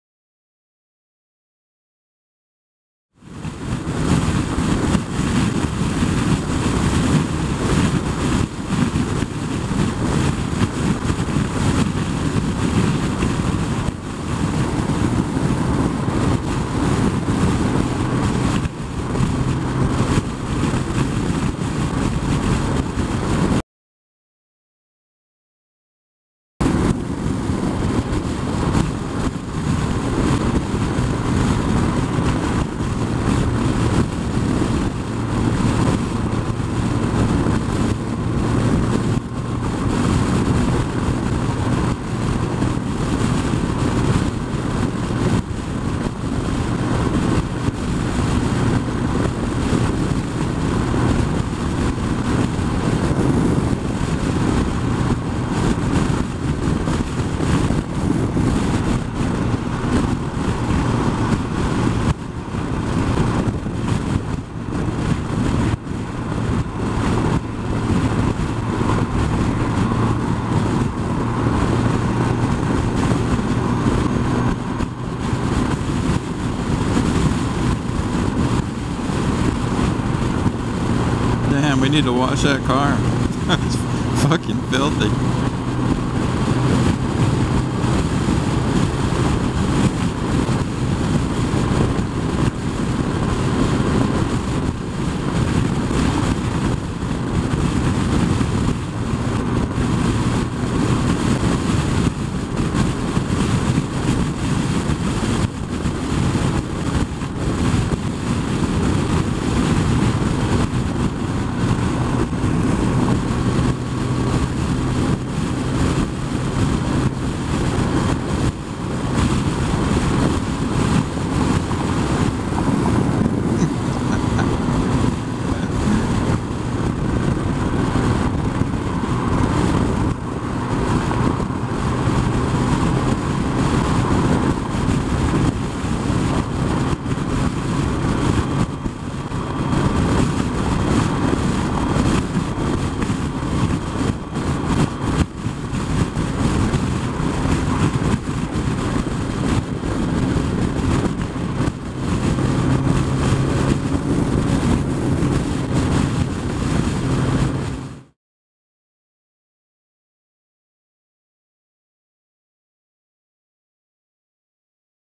Theres My Girl
Ran up on my girl on the freeway. Camera Drift Ghost